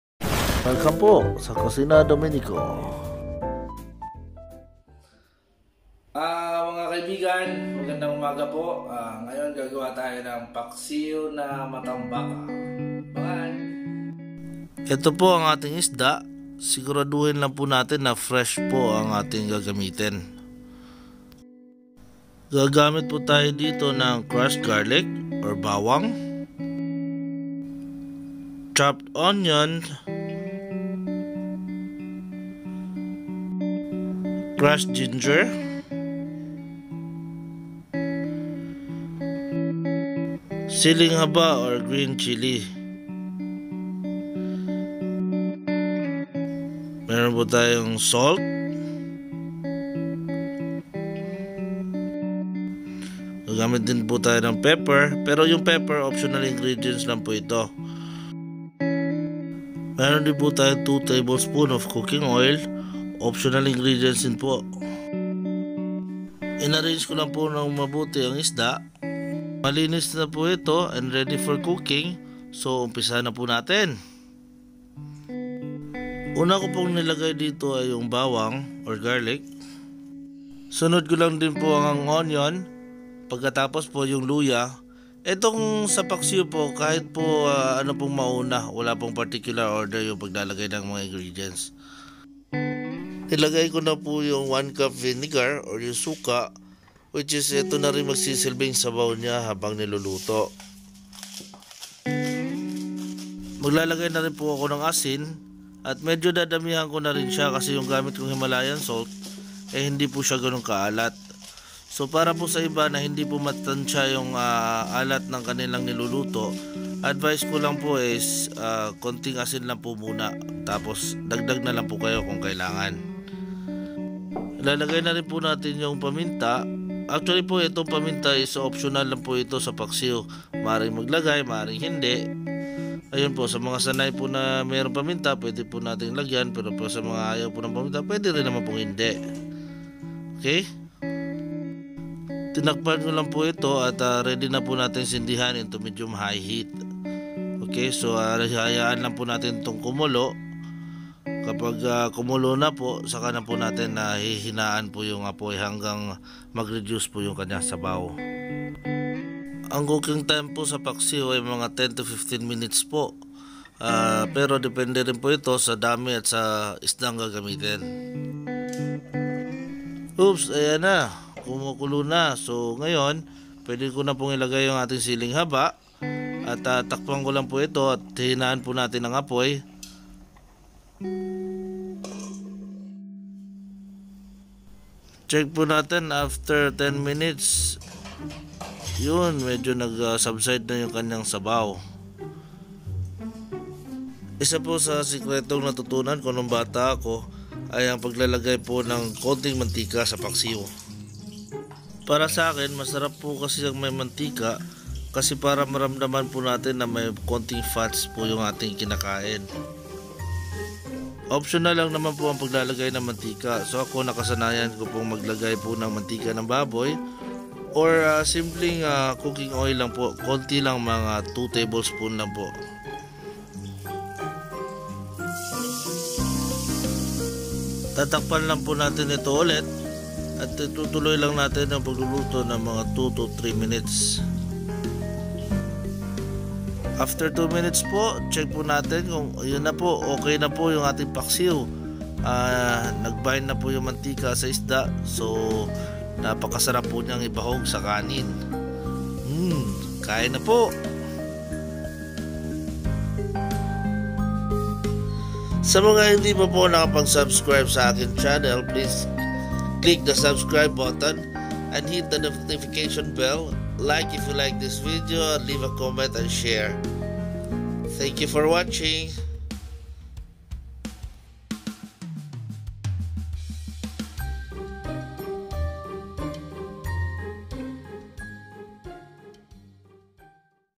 PAKSIW NA ISDA by Kusina Dominico
Paksiw is a Filipino comfort food which is very easy to make. Its a way of cooking that makes the dish last for a few days..
Best eaten in the morning or during cold days...